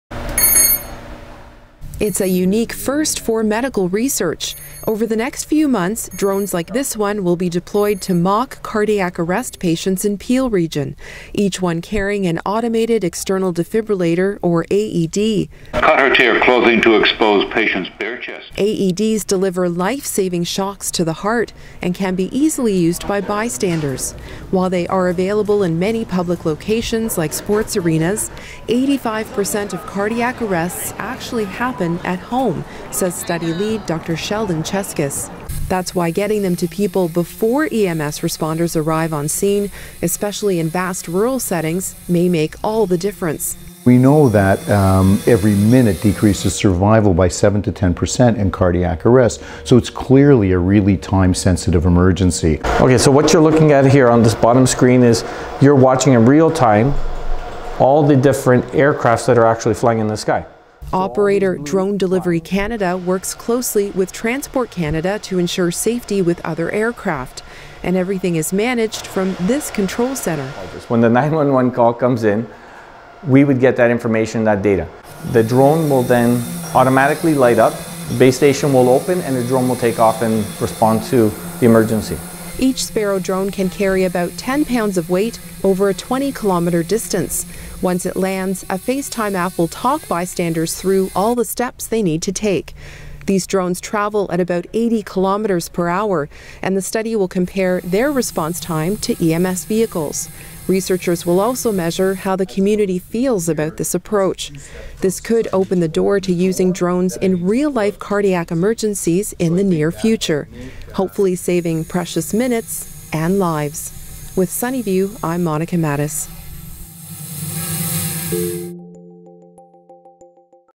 How drones could help deliver life-saving care in rural areas
Drones have long been used for recreation, but in a Canadian first, Sunnybrook is leading research to evaluate their use in responding to cardiac arrests. It’s a medical emergency where minutes matter, and there is promise that drones could help life-saving treatment begin up to 80 per cent faster.